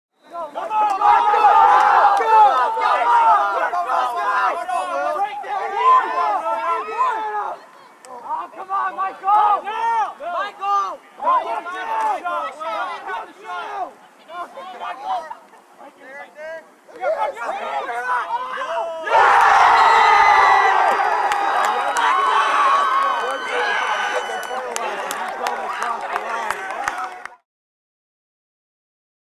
Manatee JV Lacrosse - Braveheart
After a great comeback in the second half, the game against the St Pete Green Devils came down to this Braveheart by #27 Michael Byrnes